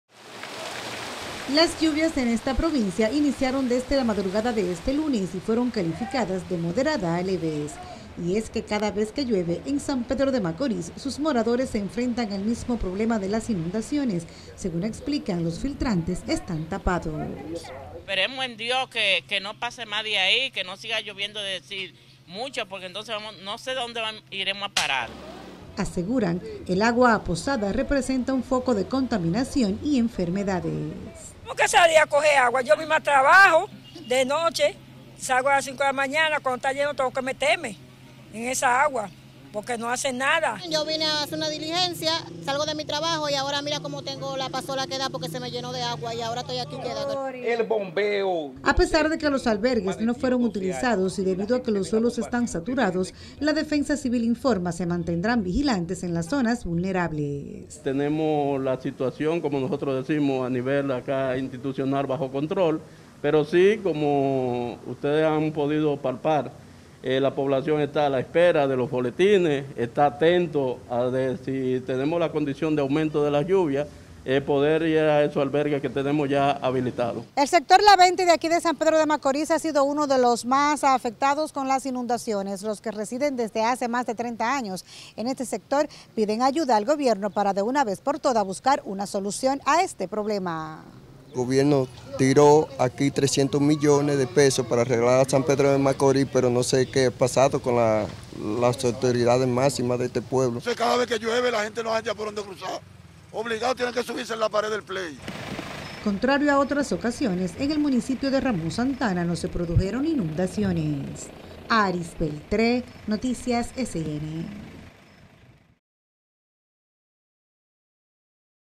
Lluvias causan daños en algunos sectores SPM
Los residentes en esa provincia aseguran que por décadas viven la misma odisea, las lluvias en esta provincia iniciaron desde la madrugada de este  lunes, considerándose  de moderadas a leves.